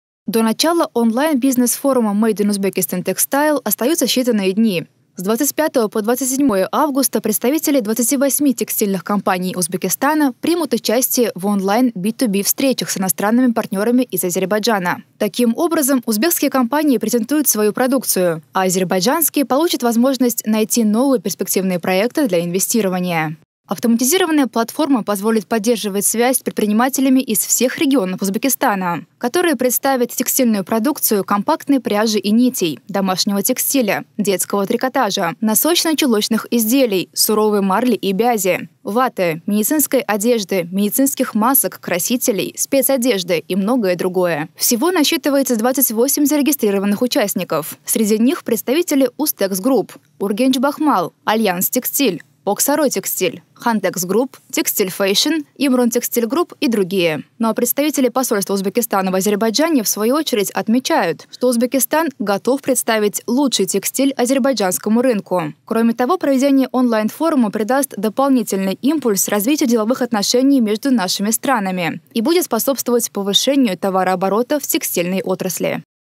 Узбекский текстиль взял курс на Азербайджан
С 25 по 27 августа представители 28 текстильных компаний Узбекистана примут участие в онлайн B2B встречах с иностранными партнерами из Азербайджана. Таким образом, узбекские компании презентуют свою продукцию, а азербайджанские – получат возможность найти новые перспективные проекты для инвестирования.